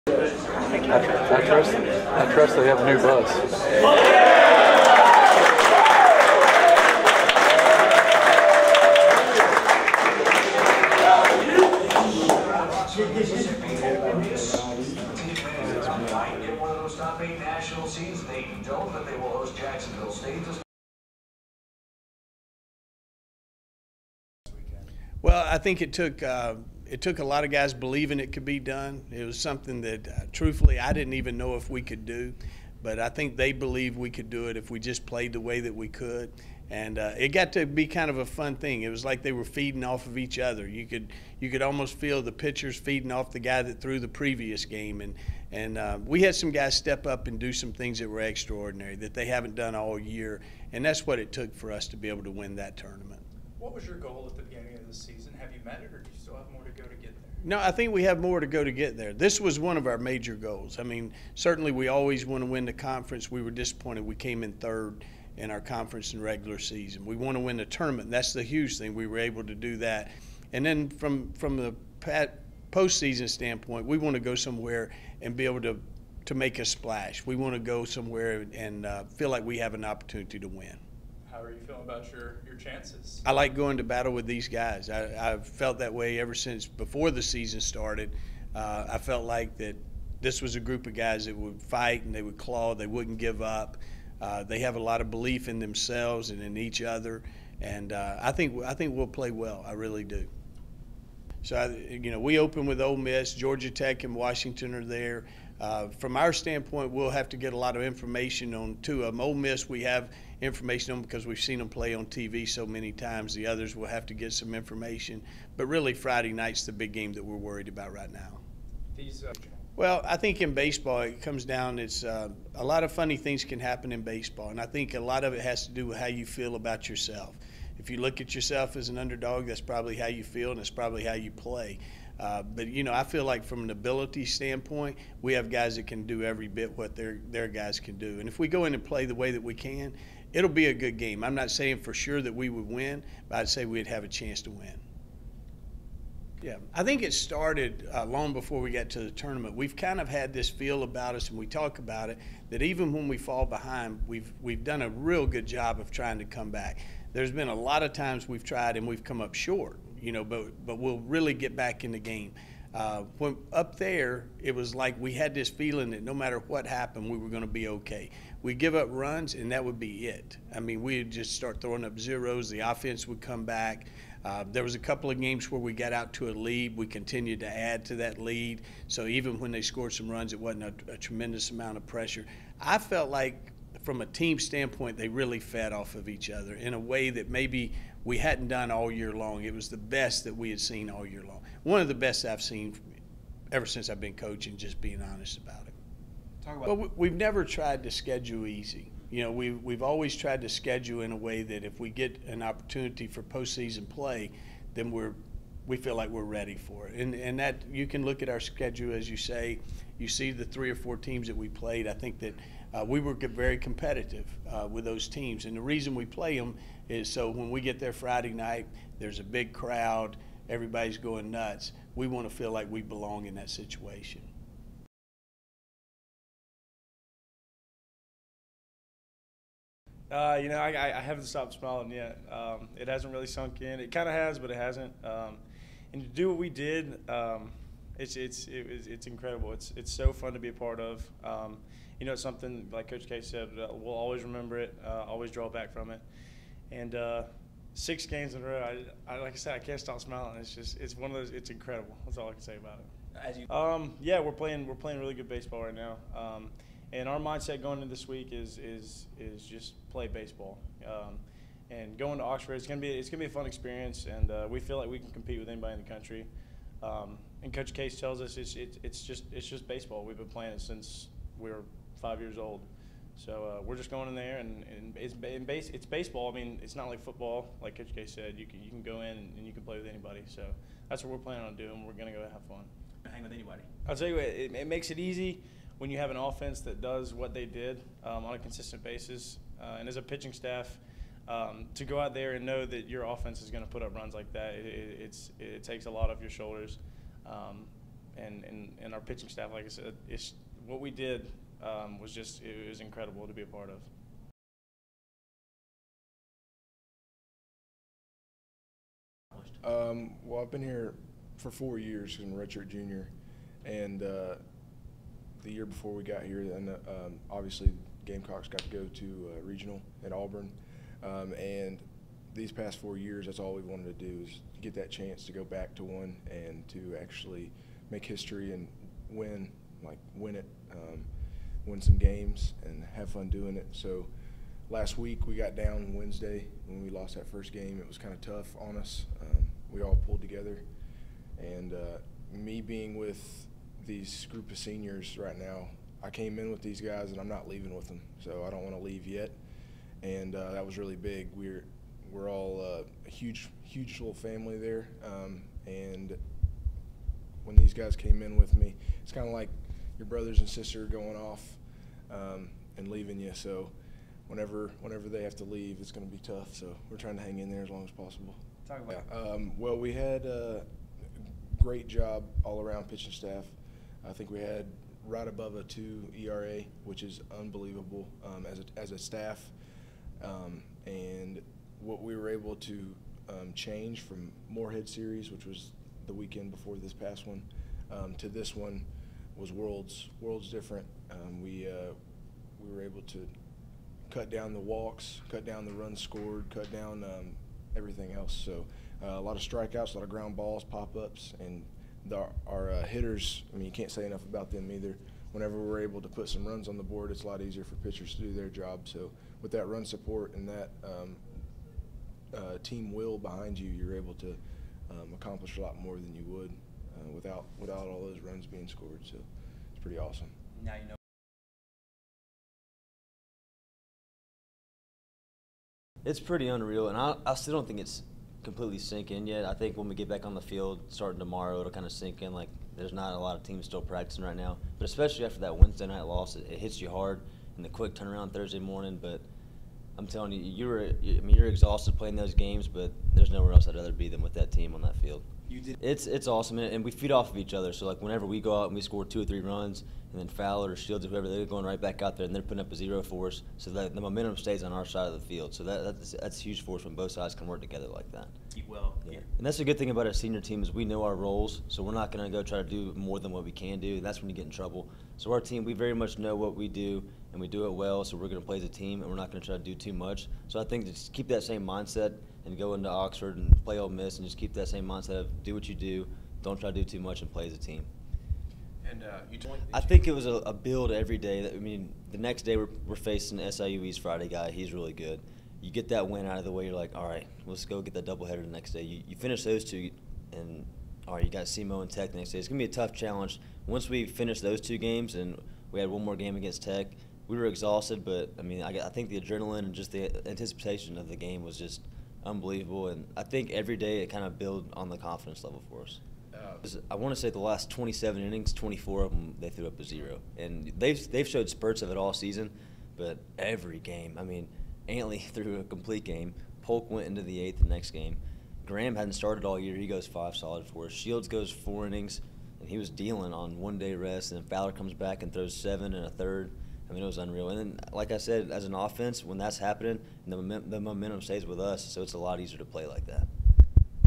2014 JSU Baseball - Selection Show Reactions
Head coach Jim Case and members of the 2014 Jacksonville State Baseball Team react to Monday's NCAA Selection Show, which matches the Gamecocks against Ole Miss in this week's NCAA Oxford Regional.